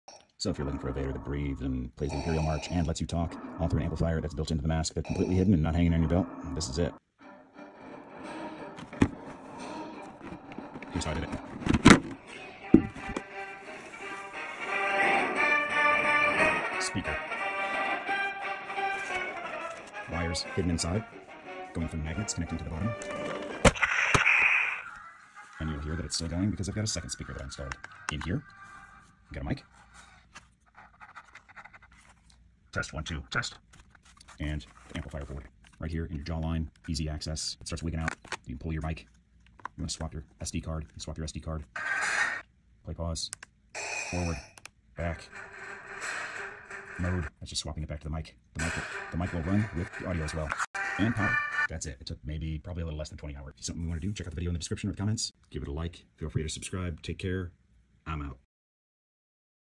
Unleash Your Inner Sith: Easy Voice Amplification for Darth Vader Black Series Helmet/mask
Added a low profile voice amplifier  & dual speakers to a Star Wars black series, Darth Vader mask.

Darth Vader mask custom amp integration using Moukey Va-1 amp low profile board.  Added 2 additional speakers - one large speaker to the to of the dome and one small exciter inside the jaw, so I’ll have amplified audio and voice with or without the top part of the mask on.

2023 = VERSION 2 of my BS Vader Amp Installs
Vader amp version 2 - overview: this video.
Vader Amp Version 2 How to video: LINK COMING SOON